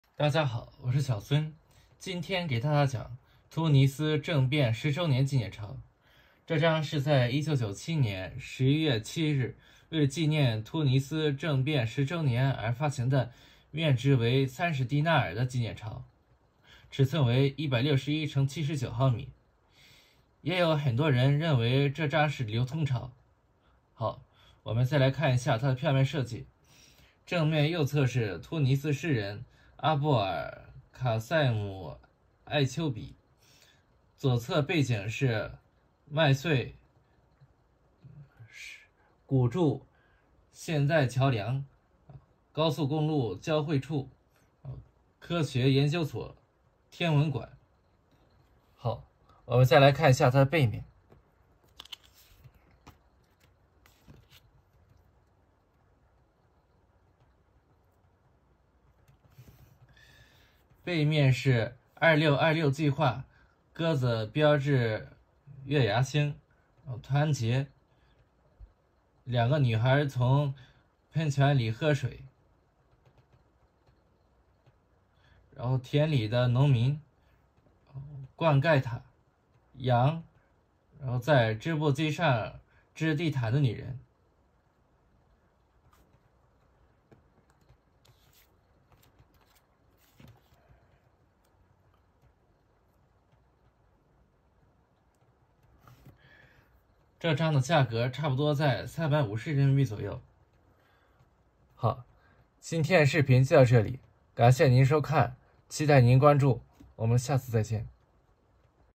突尼斯政变10周年纪念钞（Tunisia 30 Dinars commemorative banknote）
今天讲突尼斯政变10周年纪念钞！这张的面值比较特殊，是30第纳尔！也有很多人认为这张是流通钞！这张的价格差不多是350人民币左右！让我们一起来看一下吧！